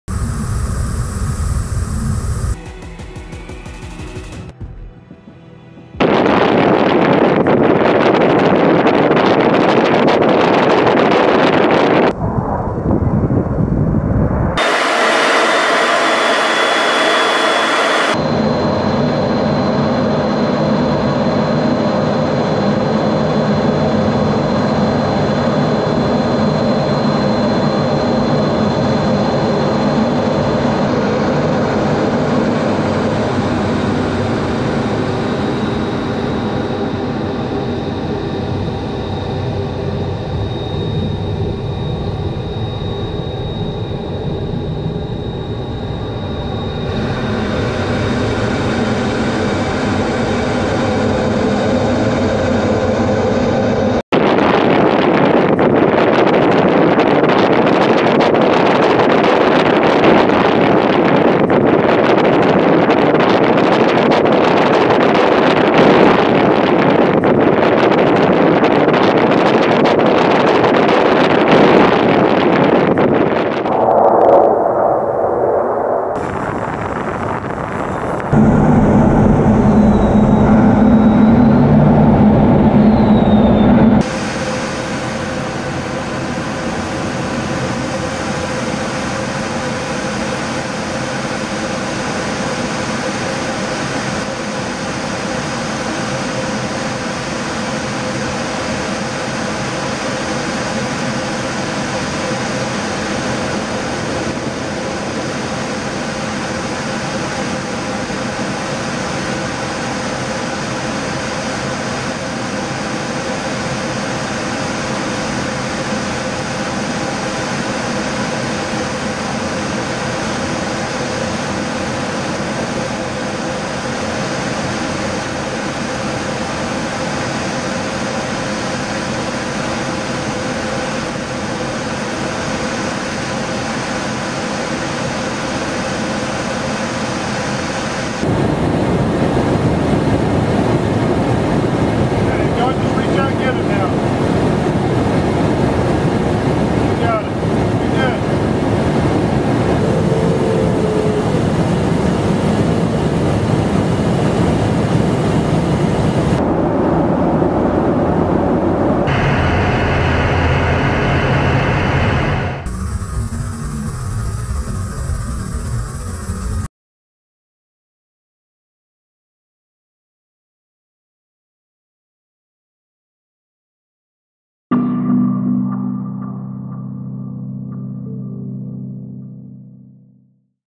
U.S. Air Force Air Demonstration Squadron "Thunderbirds"
The incredible United States Air Force Air Demonstration Squadron "Thunderbirds" b-roll.